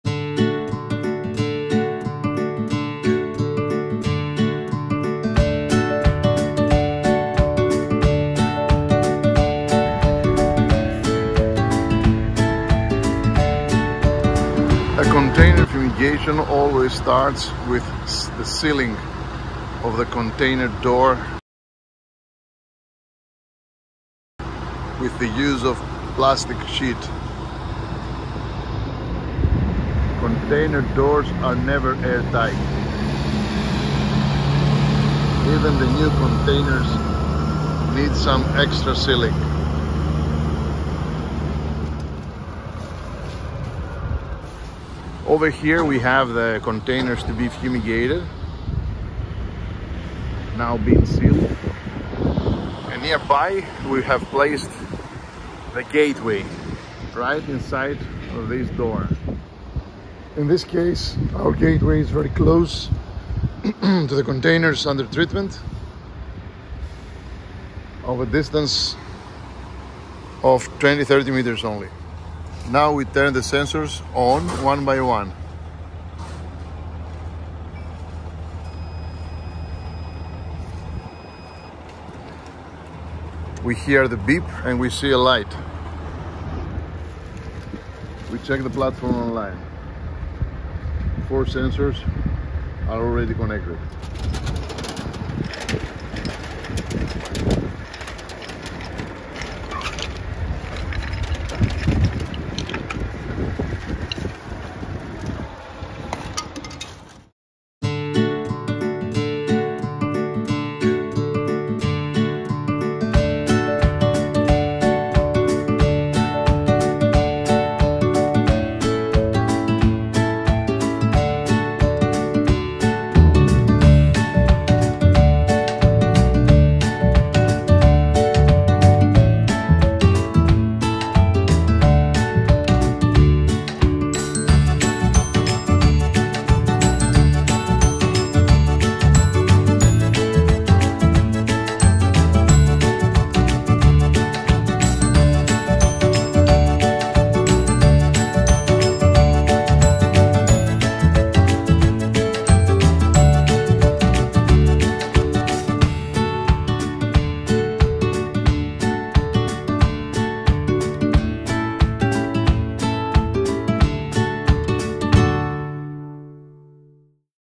Container fumigation
How to fumigate a container with phosphine. This is a step by step procedure for precision fumigation, demonstrated by AgroSpeCom fumigators and Centaur sensor manufacturers.